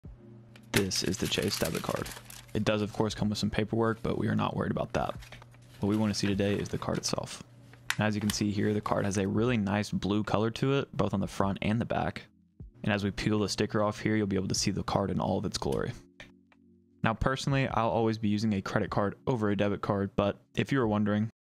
UNBOXING The Chase Debit Card 💳👀
💥 This is the unboxing of the Chase Debit card. The card itself has a really nice blue color to it both on the front and the back in classic Chase-like fashion. Even though I'll always be using a credit card over a debit card, I wanted to share this unboxing experience with you as I know many people use checking accounts from Chase for their daily money management.

The content in this video is accurate as of the posting date. Some of the offers mentioned may no longer be available. Spencer Johnson is not a financial advisor.